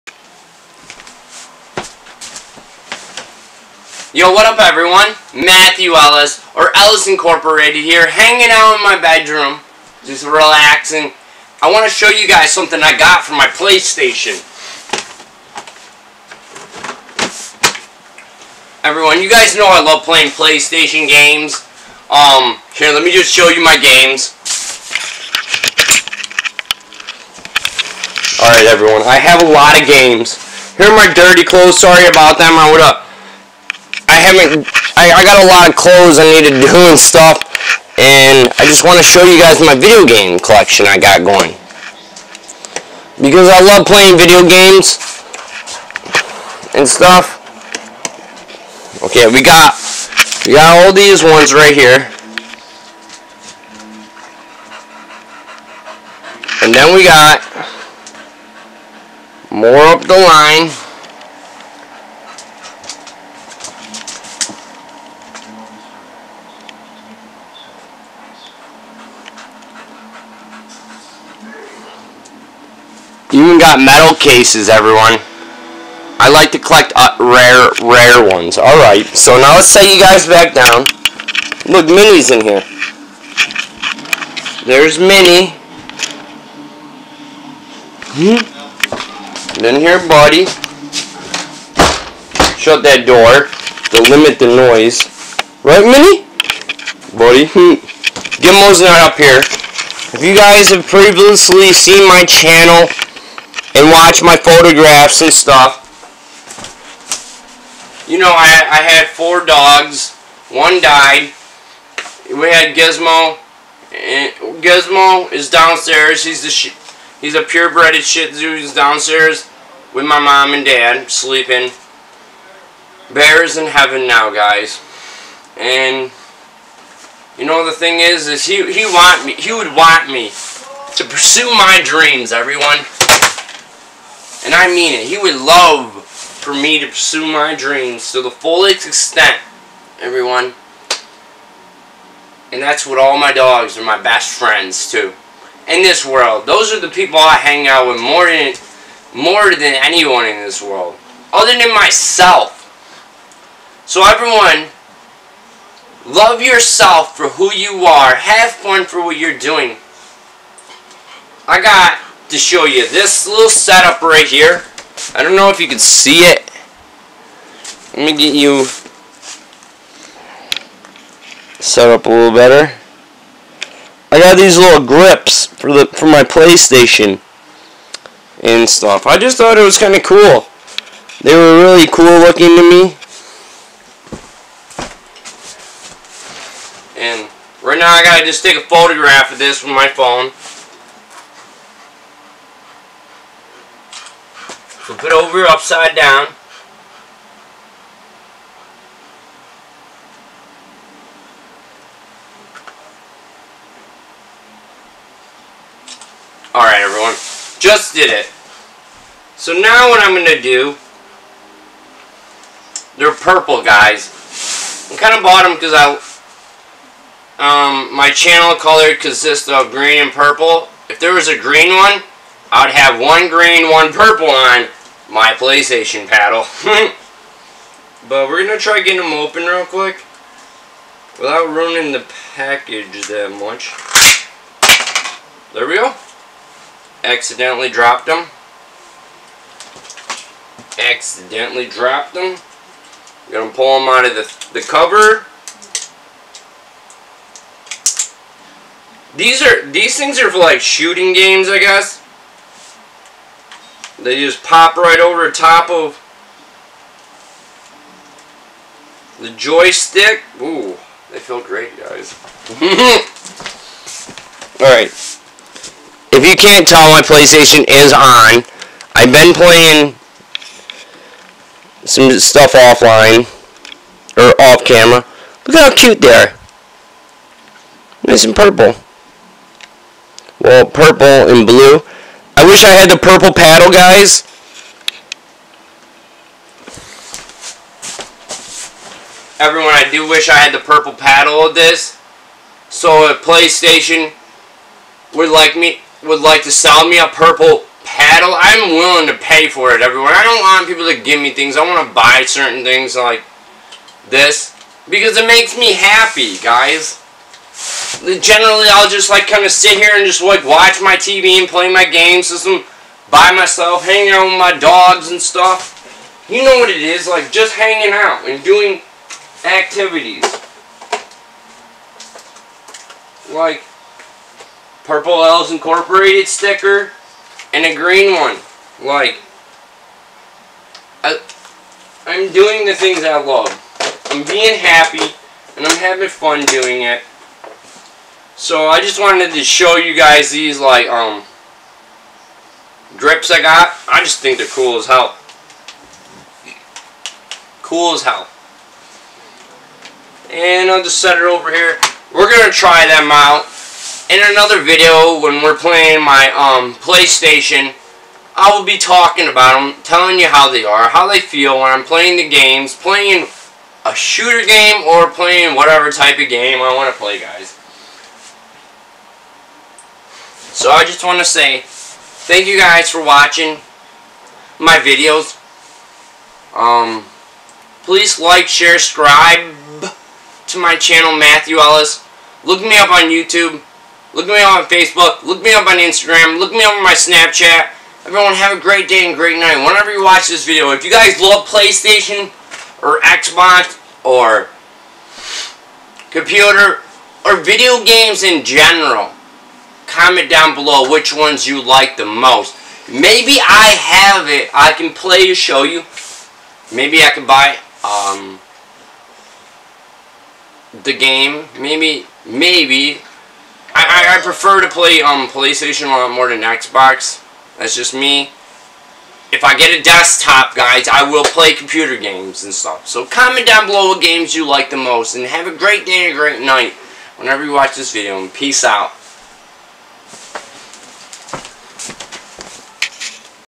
Matthew Ellis or Ellis Incorporated I want to show you all my new ps4 Fpsfreek Galaxy
Matthew Ellis or Ellis Incorporated I want to show you all my new ps4 Fpsfreek Galaxy i like to play videos games for fun and to be happy and i am doing what i like to do for fun and to be happy for the rest of my life